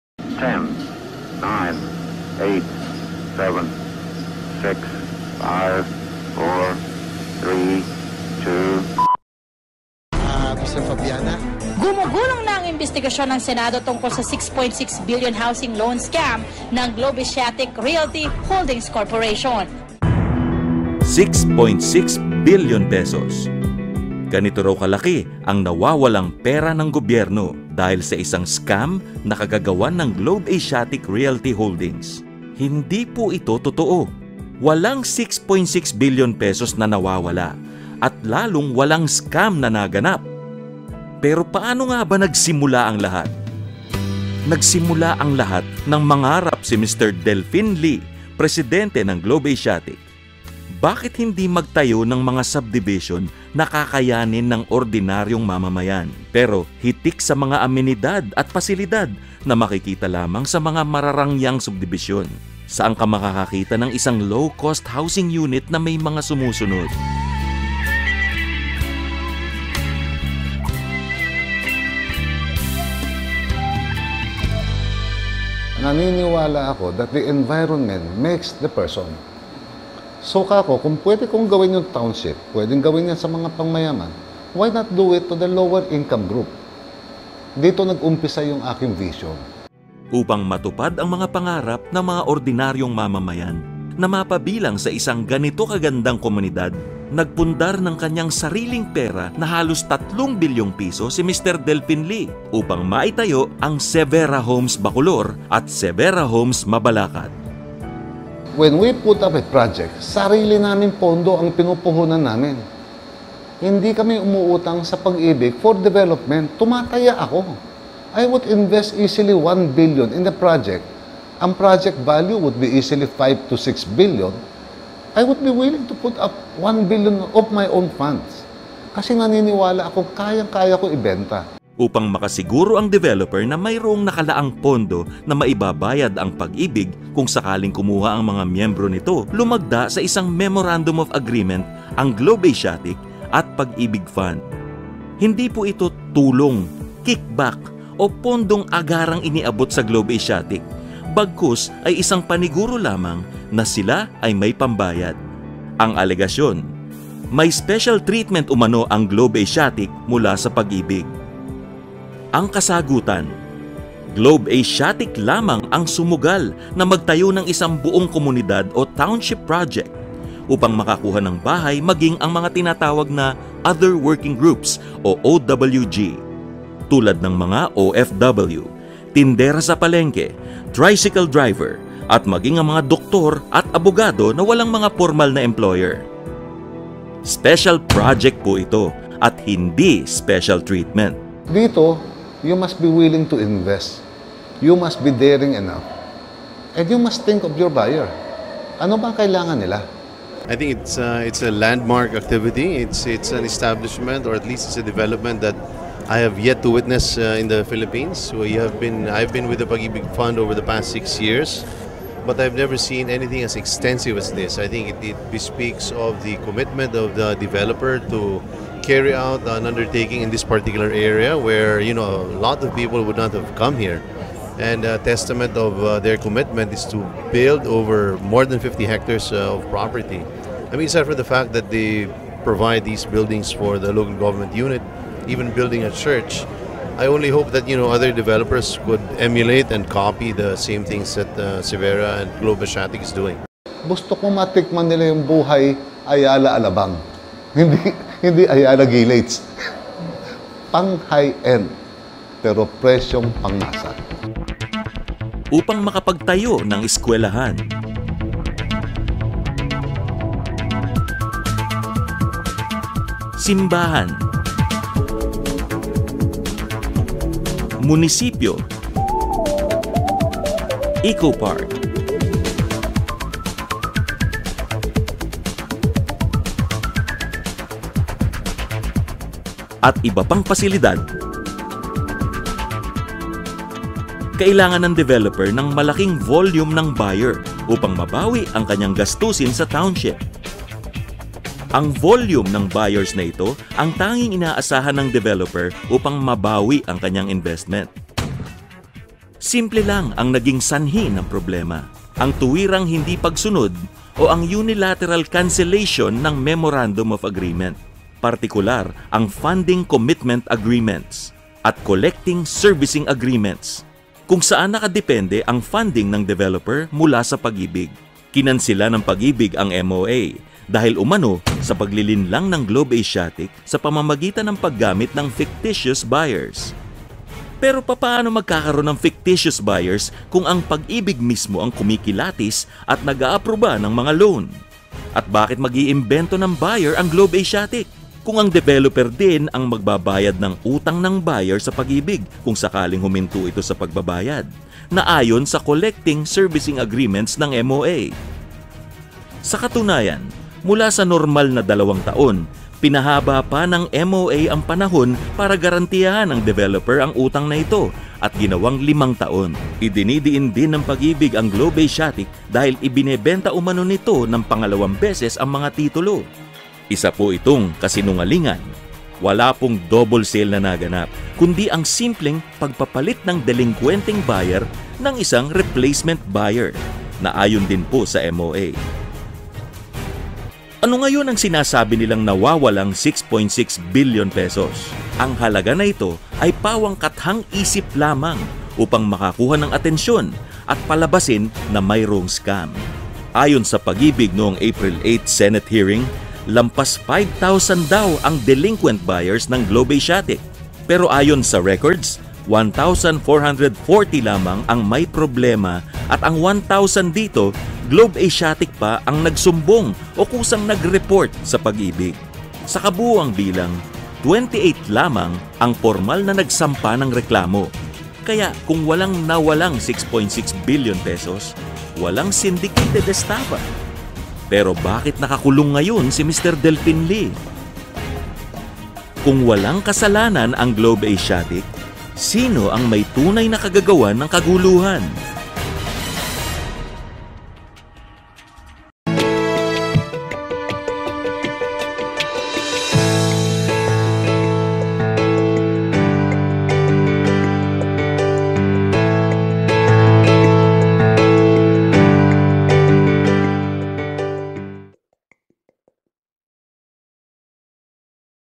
Ang Katotohanan sa Pagibig Fund Scam Part 1 of 4
Gumugulong na ang imbestigasyon ng senado tungkol sa 6.6 Billion housing loan scam ng Globe Asiatique Realty Holdings Corporation.
6.6 Billion pesos, ganyan raw kalaki ang nawawalang pera ng gobyerno dahil sa isang scam na kagagawan ng Globe Asiatique Realty Holdings. Hindi po ito totoo. Walang 6.6 Billion pesos na nawawala at lalong walang scam na naganap. Pero paano ng aba nagsimula ang lahat?
Nagsimula ang lahat na mangarap si Mr. Delfin Lee, presidente ng Globe Asiatique. Bakit hindi magtayo ng mga subdivision na kakayanin ng mga ordinaryong mamamayan pero hitik sa mga amenidad at pasilidad na makikita lamang sa mga mararangyang subdivision.

For more information.Follow us at globeasiatique.ph and delfinlee.com the official website of Delfin Lee.